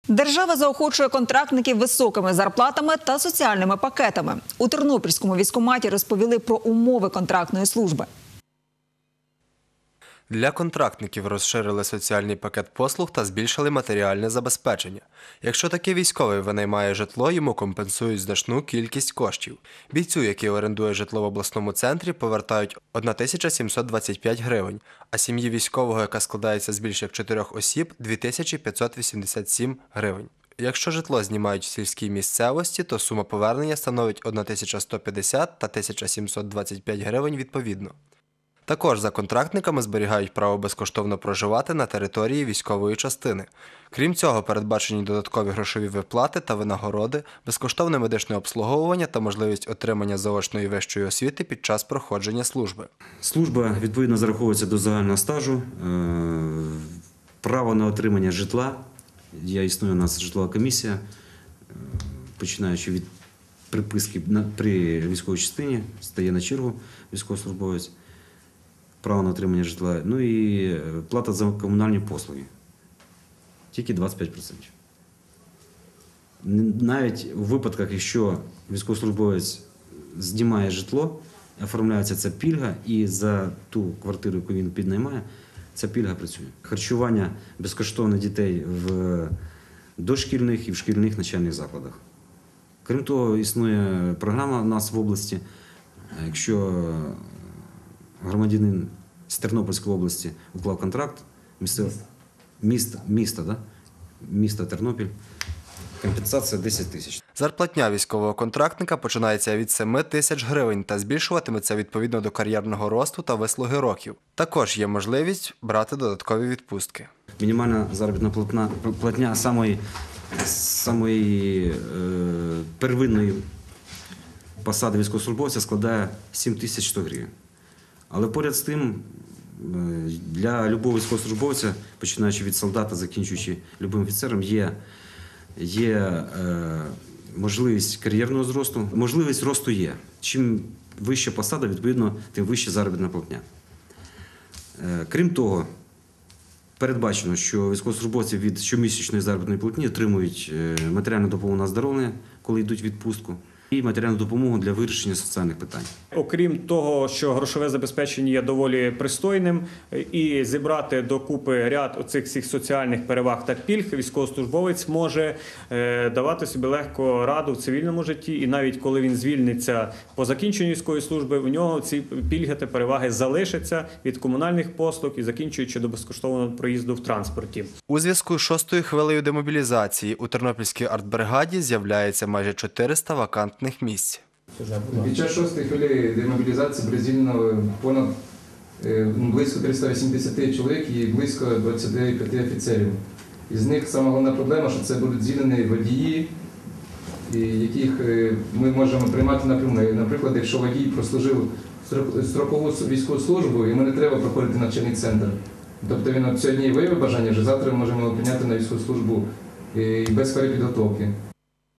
Військовим-контрактникам компенсуватимуть оренду житла
Військовим-контрактникам компенсуватимуть кошти за наймане житло. Про грошове забезпечення та інші переваги військової служби за контрактом розповіли у Тернопільській окремій артилерійській бригаді.